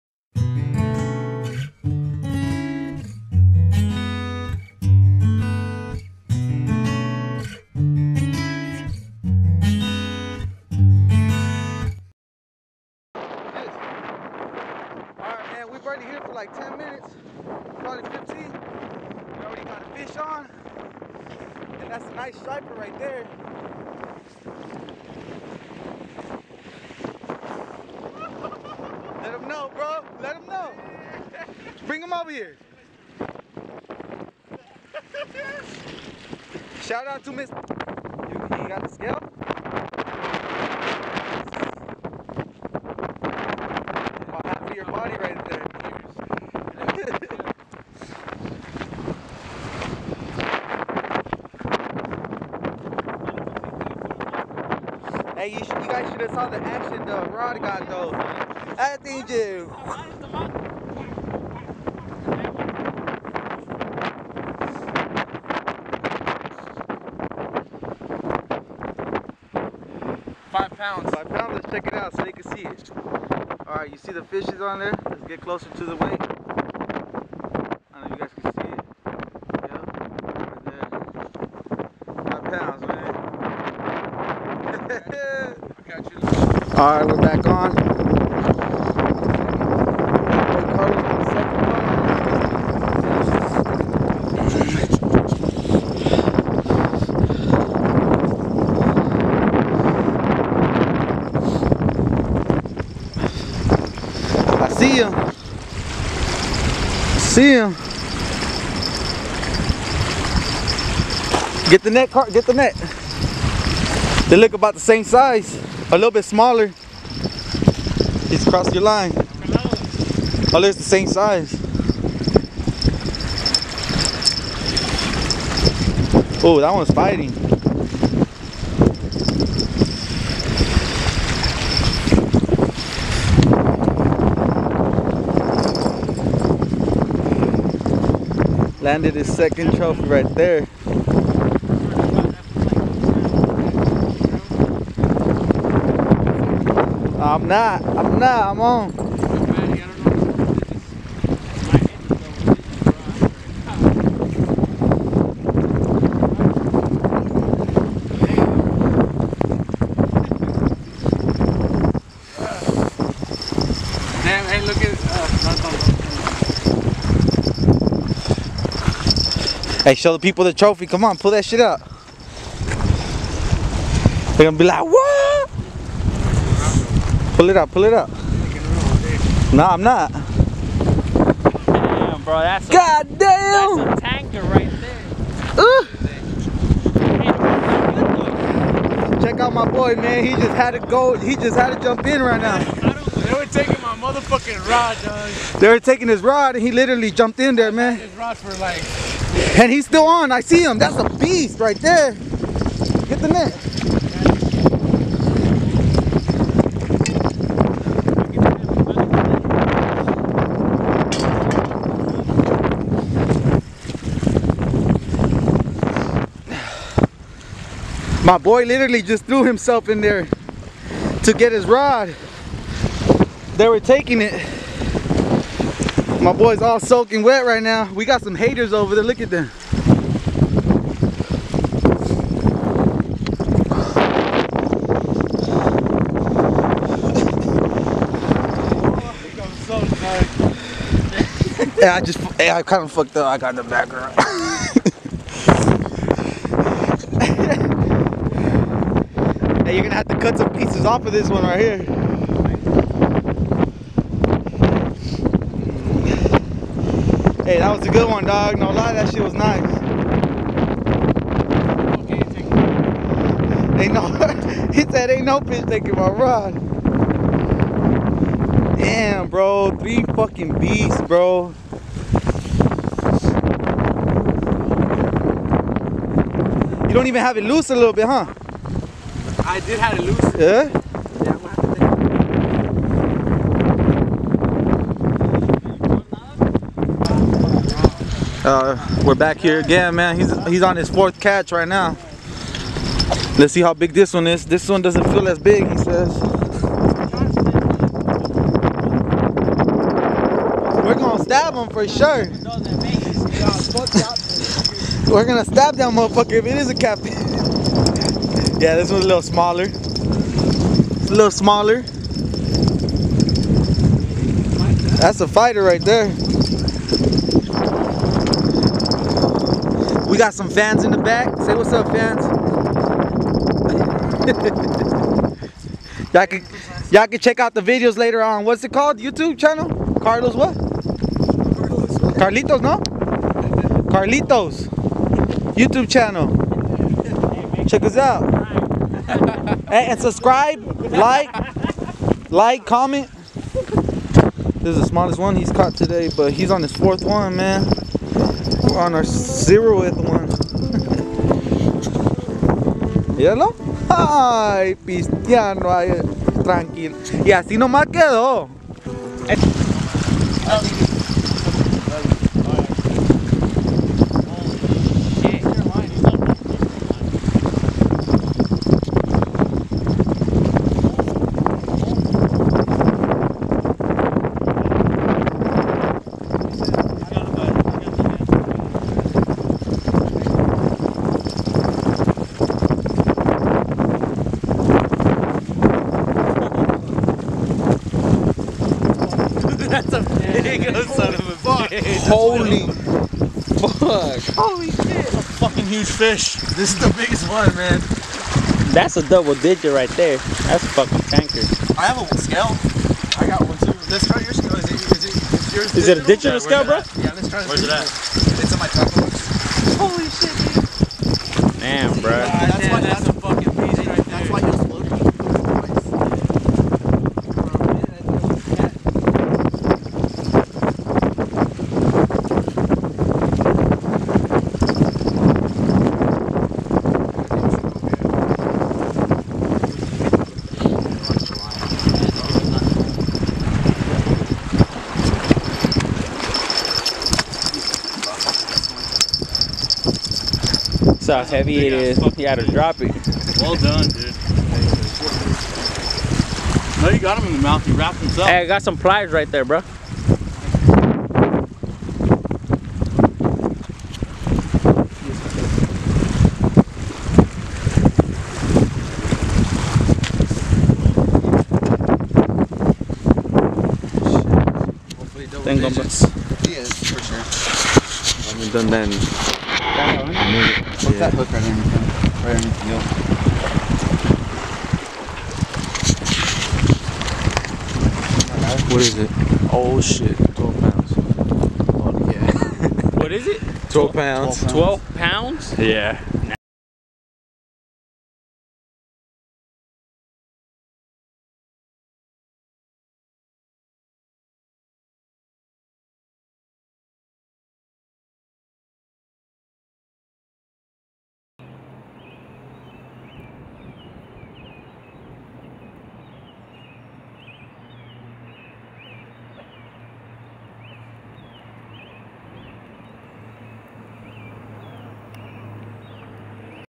Lake Mohave Striper Fishing 12 pounder!
Windy day at Lake Mohave. still managed to catch some fish! caught a 12 pound striper, fishing on anchovies . Catch me fishing.

MORE FISHING&HUNTING VIDEOS BELOW